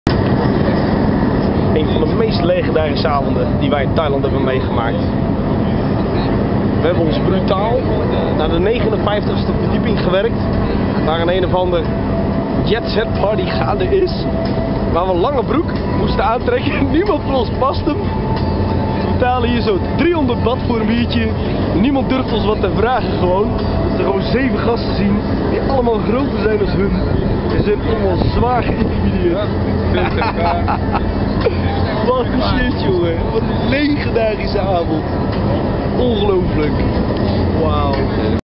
on top of the Banyan Tree Hotel in Bangkok, Thailand
we bluffed our way in to a jetsetparty posing as the dutch national VOLLEYBAL-team (!) on our way to the olympics :D